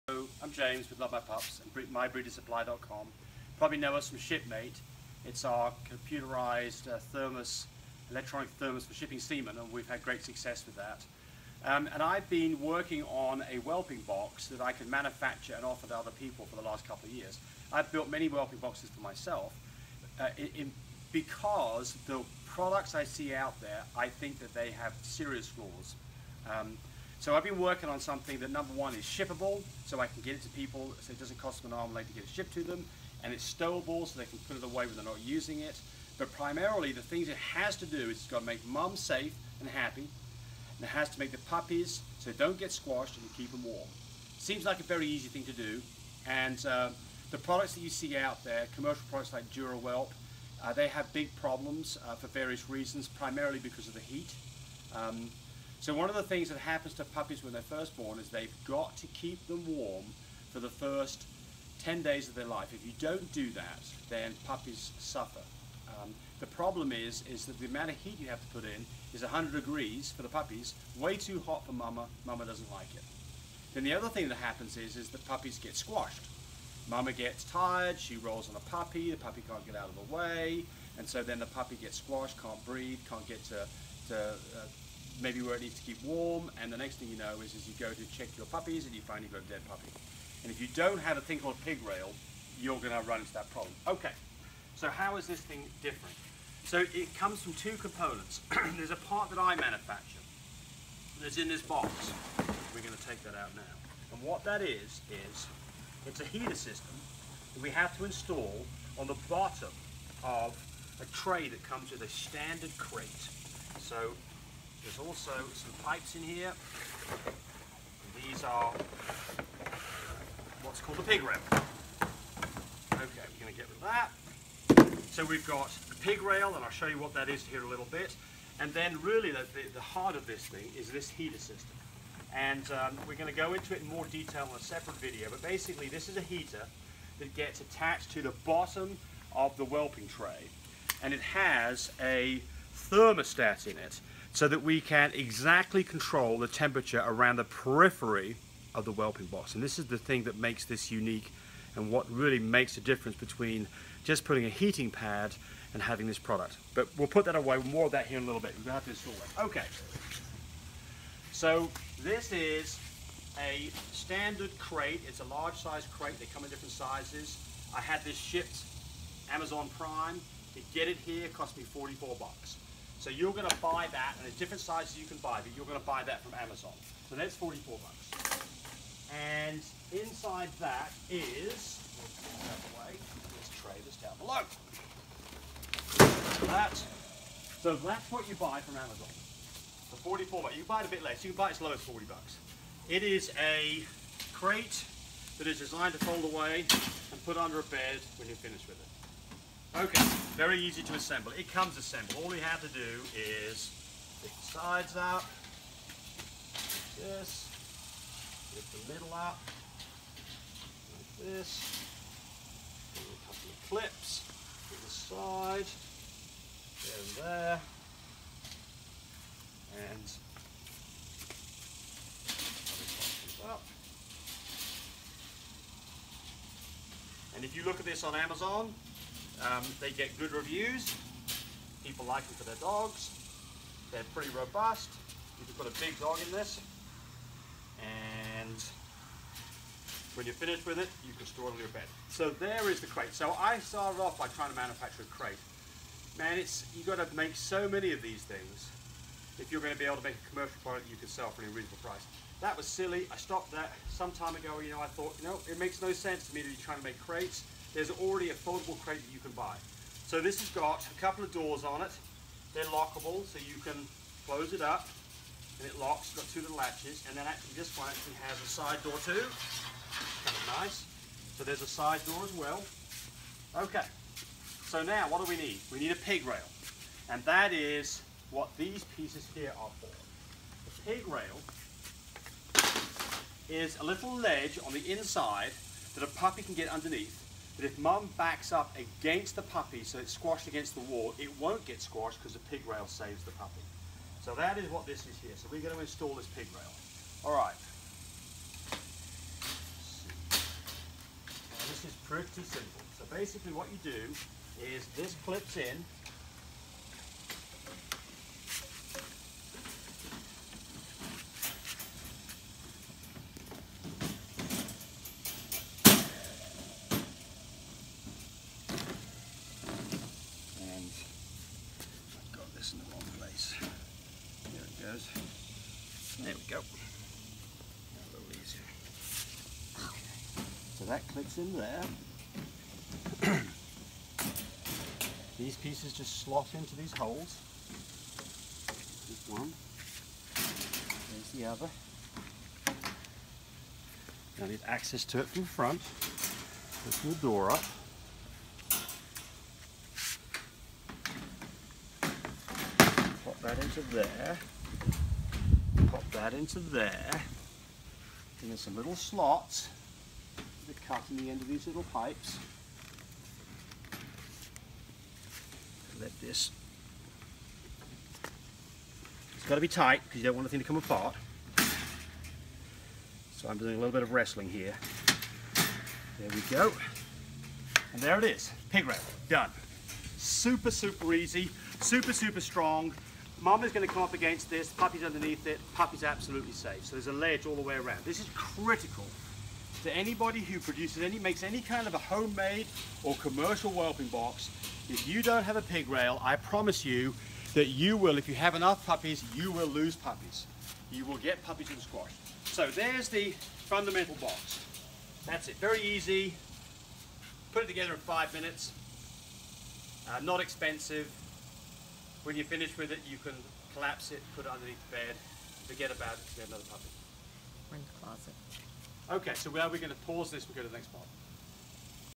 Puppy whelping box Part I
Novel whelping box  solves two major problems
Applying heat where it is needed
And providing a pig rail to keep puppies safe
assembles in minutes with out tools
Using the whelping box very safe environment keeps puppies both safe and warm
Im selling the first 100 for $149 plus $15 ship  Then the price goes up to $249
You get the perimter heater
The digital thermostat
Snap in pig Rails
You will need a 42" Crate
you can get one including delivery from Amazon prime
$44 Oxgord 42" Crate
I can custom build one but i will need inside dimensions of our cage
Patent Pending